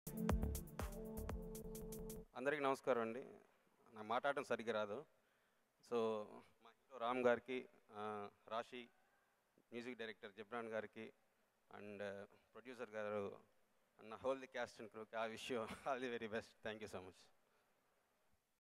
Director Kishore Tirumala Speech at HYPER Movie Theatrical Trailer Launch - Ram, Raashi Khanna
Hyper(Prathi Intlo Okaduntaadu)  Movie Audio Launch Grand Live Event is happening today at JRC function hall Hyderabad.

Starring : Ram, Raashi Khanna, Satya Raj, Rao Ramesh, Murali Sharma, Posani Krishna Murali, Prabhas Sreenu, Tulasi, Hema, Priya
DOP : Sameera Reddy
Art : Avinash Kolla
Editor : Gowtham Raju
Dialogues : Abburi Ravi
Producers : Ram Achanta, Gopi Achanta and Anil Sunkara
Director : Santosh Srinivas